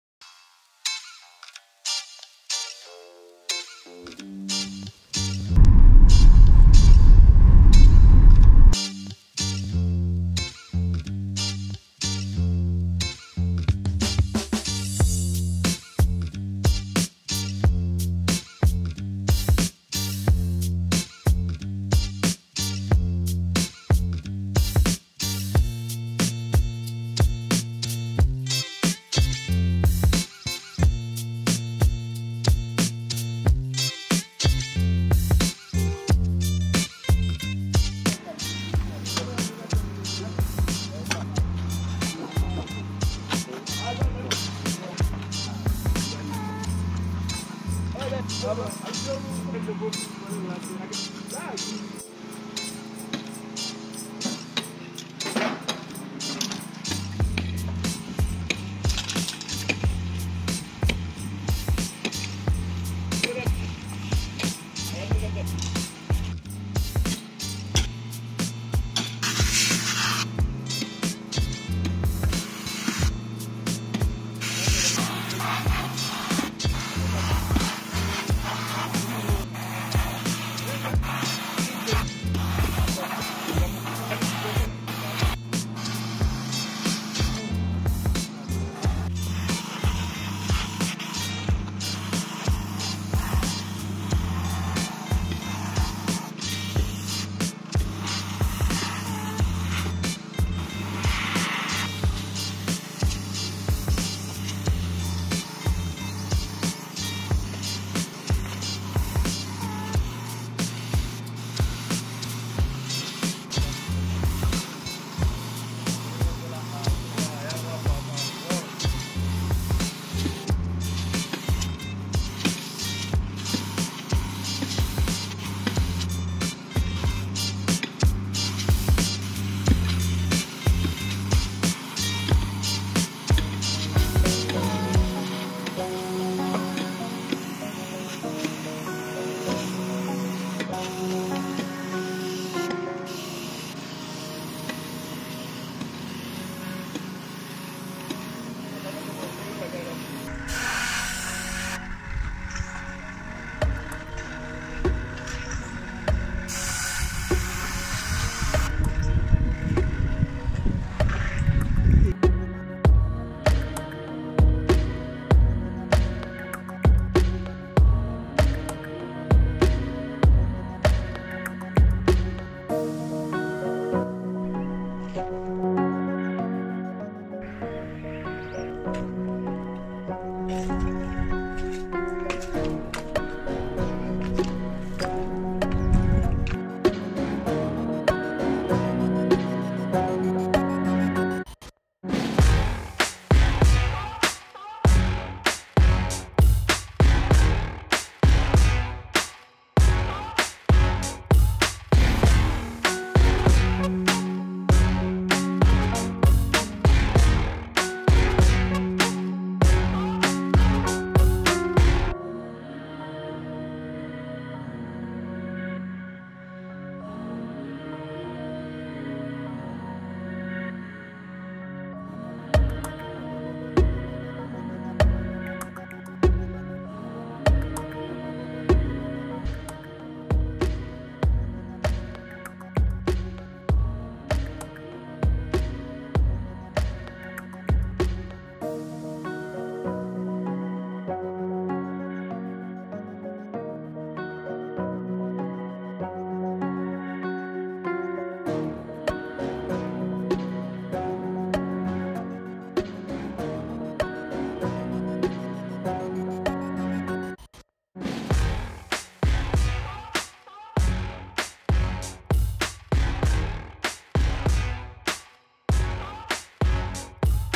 Graffiti & Airbrush (wild life Mural) | time lapse weekend!
Spraying an Orchid and Wild Life Mural for a Gender Based Violence Center! using spray guns, anest iwata airbrush and Graffiti to get this done in time. also some adventuring :D

Like the vibe?